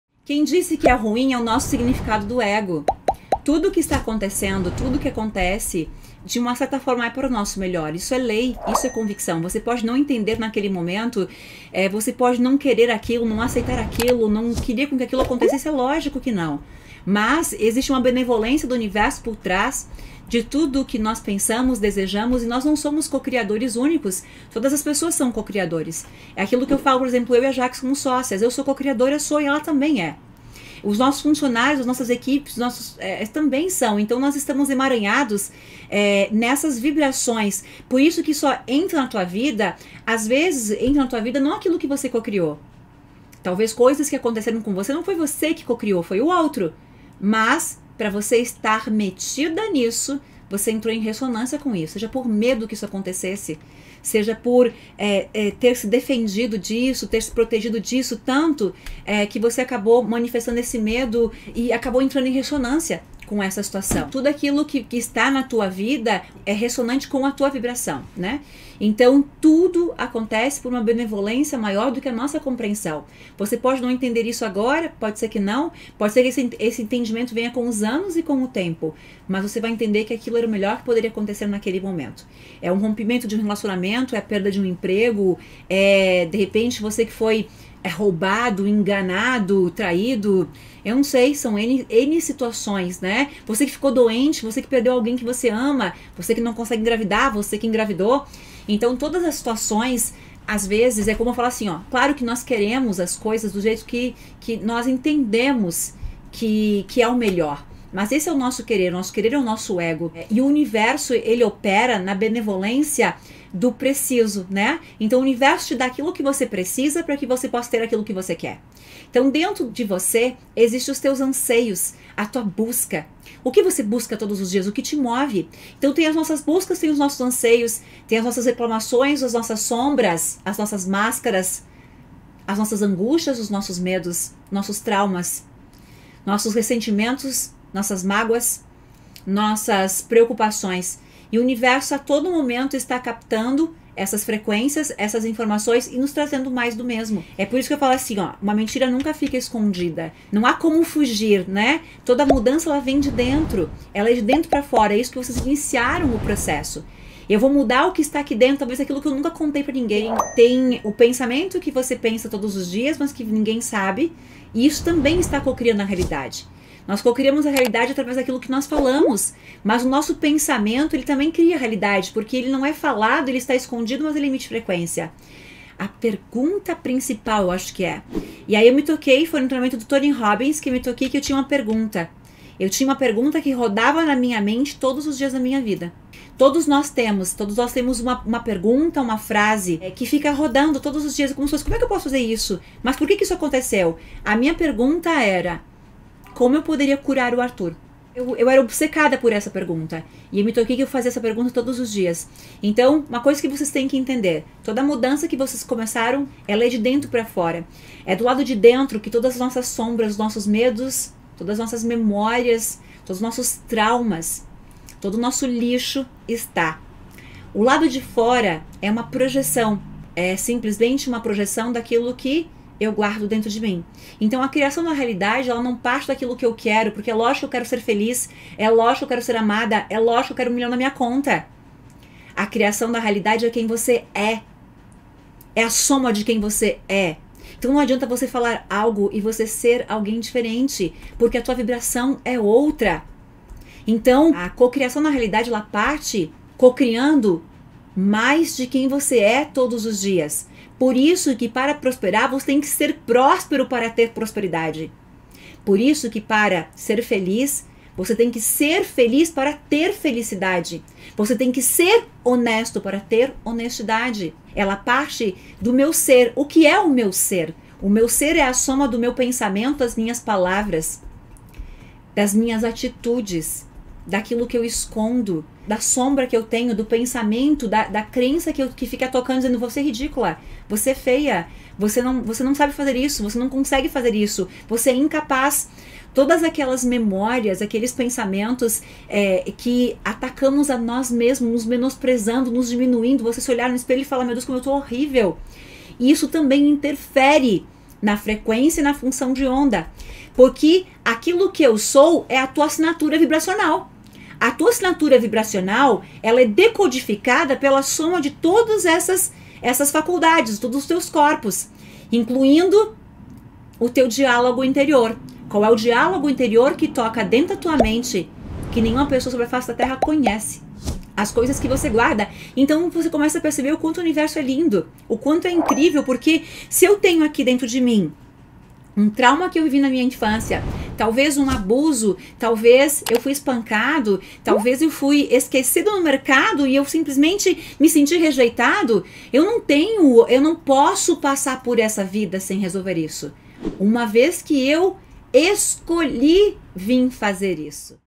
Toda mudança vem de dentro | Elainne Ourives
Amores

No vídeo de hoje, falaremos sobre as mudanças que queremos fazer, muitos pensam que ela acontece de fora pra dentro mas isso não é o que ocorre

Precisamos limpar nosso lixo, curar nossos medos e crenças

Conta nos comentários, o que achou